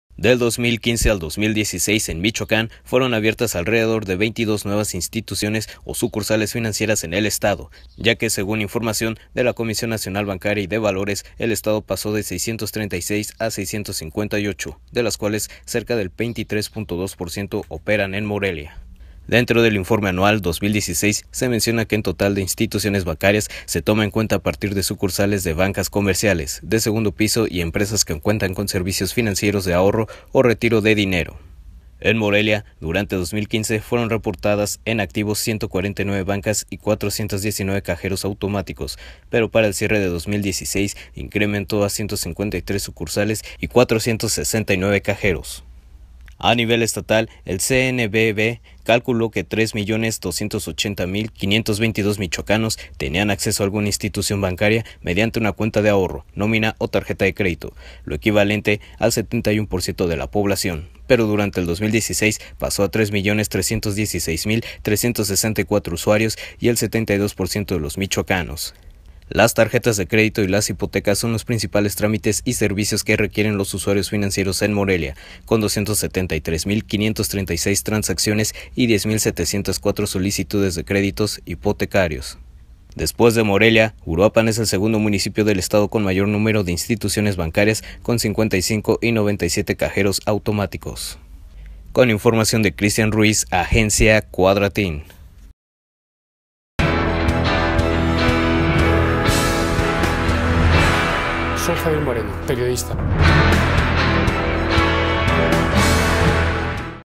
Abrió Michoacán 22 sucursales financieras de 2015 al 2016
MORELIA, Mich., 23 de junio de 2017.- Del 2015 al 2016 en Michoacán fueron abiertas alrededor de 22 nuevas instituciones o sucursales financieras en el estado, ya que según información de la Comisión Nacional Bancaria y de Valores (CNBV), el estado pasó de 636 a 658, de las cuales cerca del 23.2 por ciento operan en Morelia.